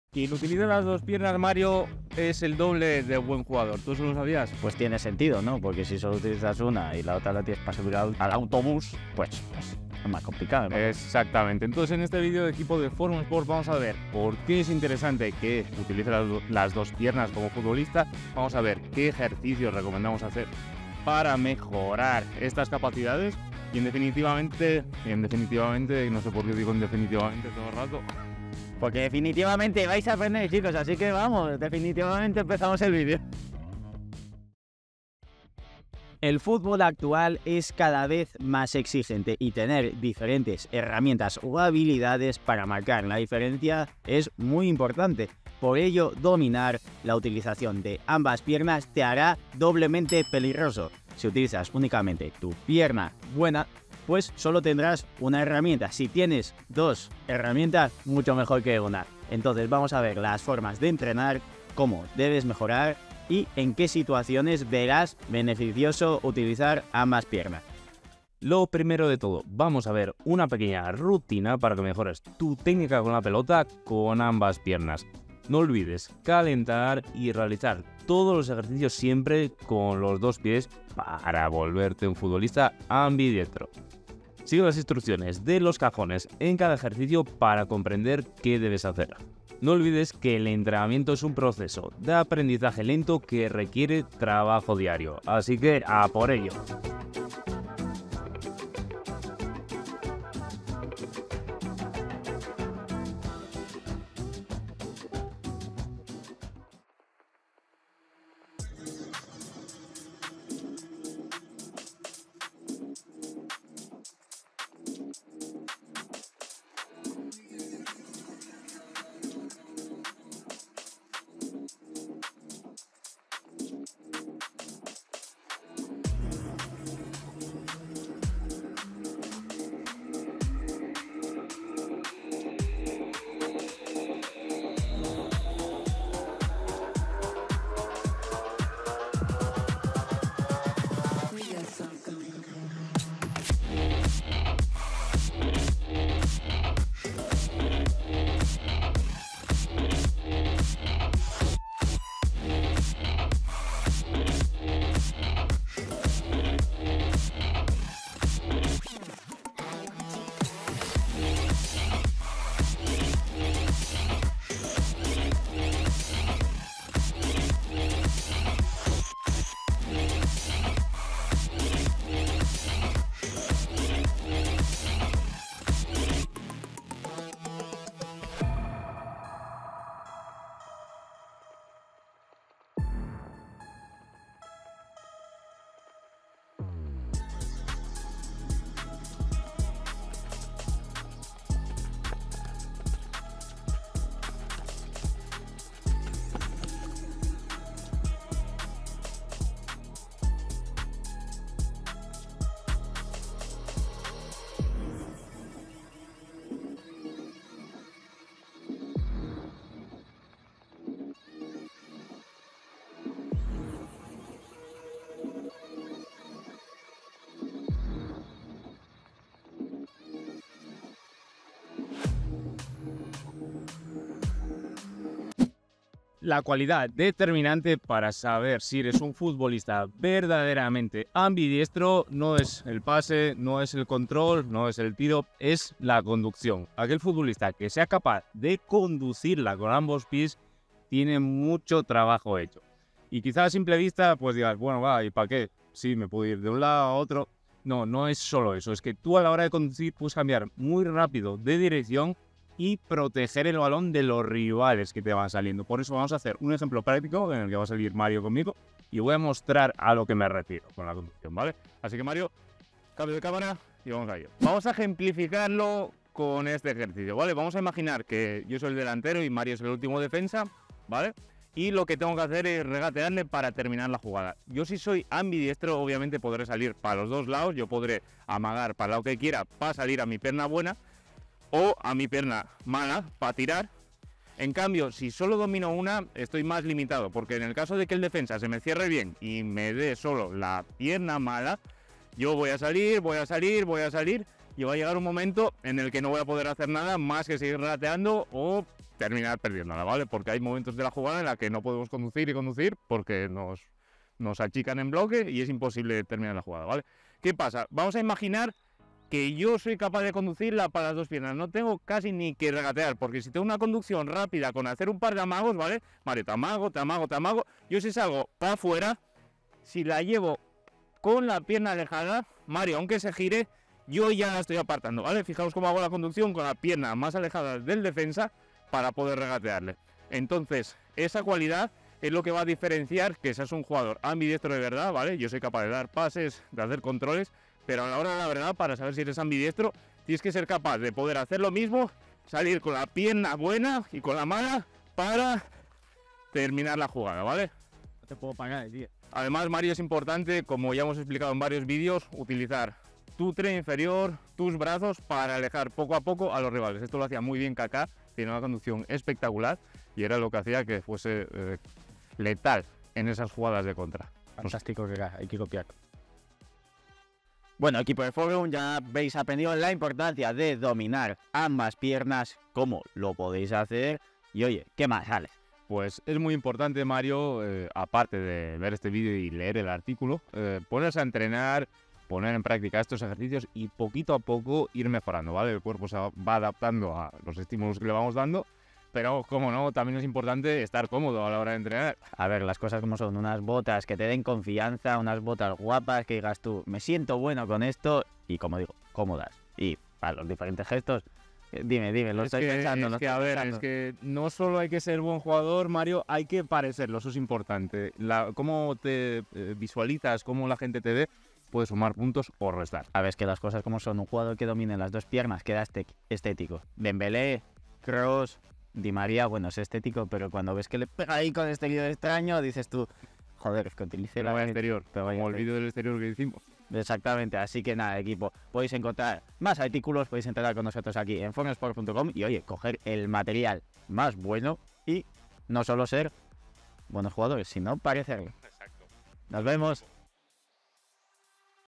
Cómo ser ambidiestro en fútbol: Ejercicios y beneficios
Aprende las técnicas esenciales para ser un futbolista ambidiestro y tener un total dominio de ambas piernas en el fútbol.